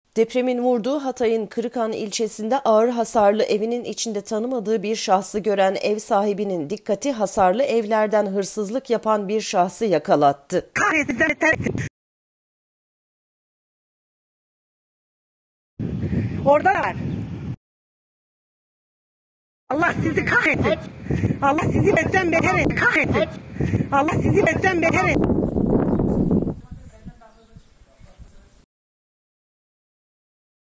EV SAHİBİNİN DİKKATİ ENKAZ FARESİNİ YAKALATTI
Depremin vurduğu Hatay’ın Kırıkhan ilçesinde ağır hasarlı evinin içinde tanımadığı 1 şahsı gören ev sahibinin dikkati, hasarlı evlerden hırsızlık yapan 1 şahsı yakalattı.